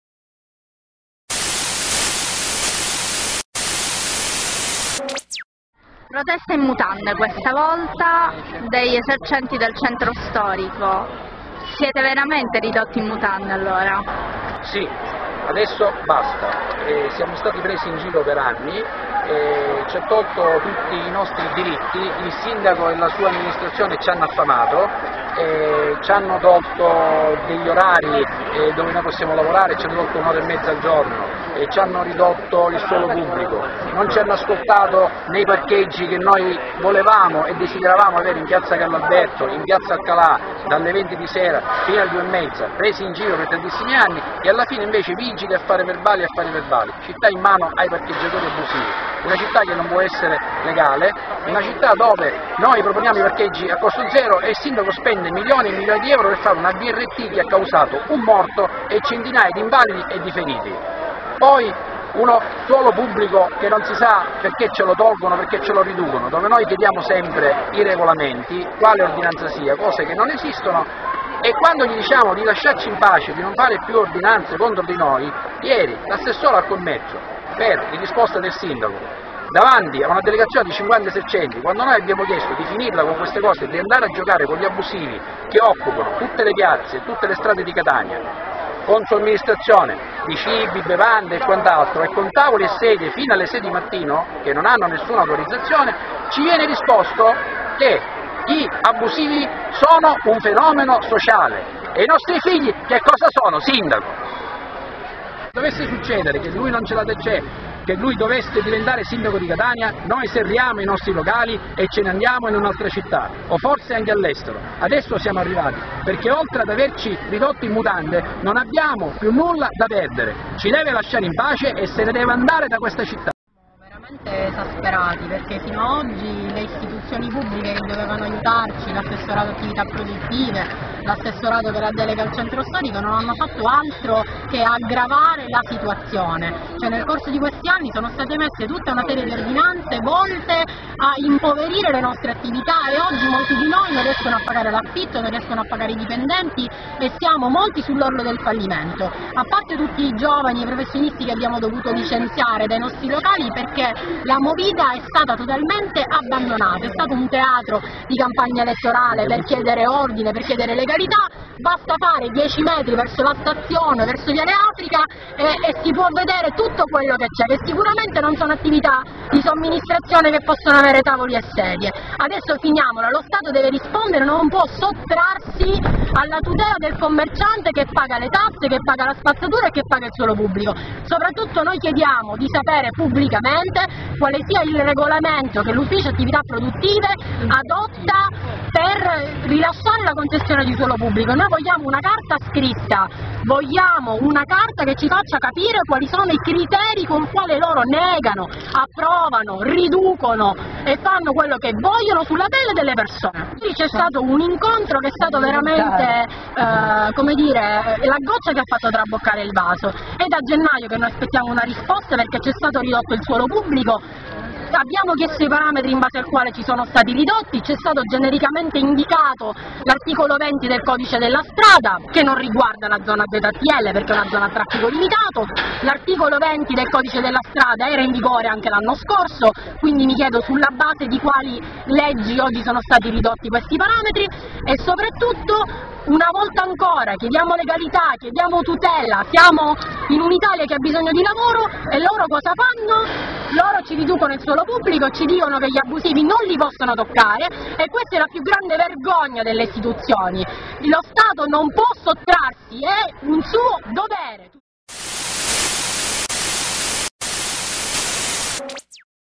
Gli esercenti in mutande
I locali del centro storico sono al collasso, oltre il 20% ha già chiuso e il restante rischia la chiusura